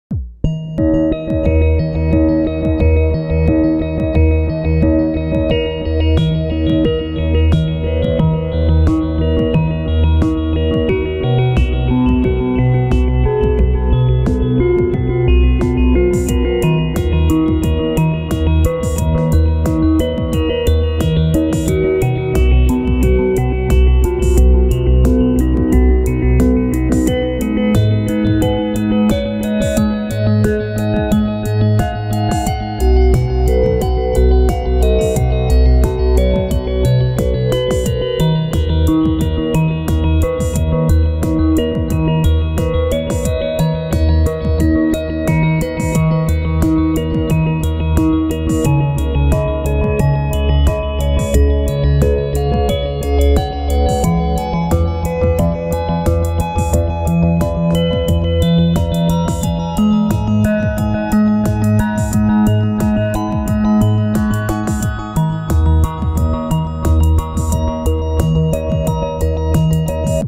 Verbos Analog Shift Register
While playing a simple arpeggio, two Verbos Harmonic Oscillators are "animated" with CVs coming out of the Analog Shift Register from a Verbos Random Sampling module. Audio then goes through the DPW Design Mög module and 4MS Dual Looping Delay.